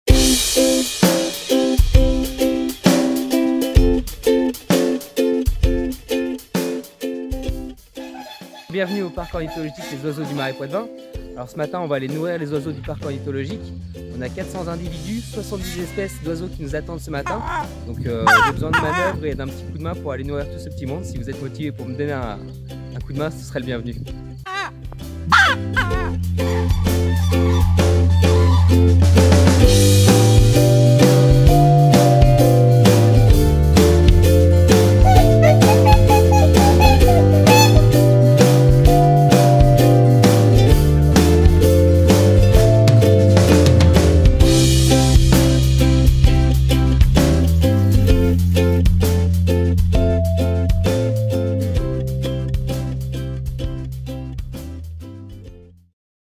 Ils ont testé pour vous !
Soigner les oiseaux, découvrir le métier de soigneur animalier, les coulisses du Parc des Oiseaux du Marais poitevin... une expérience exceptionnelle pour vivre le Marais poitevin autrement.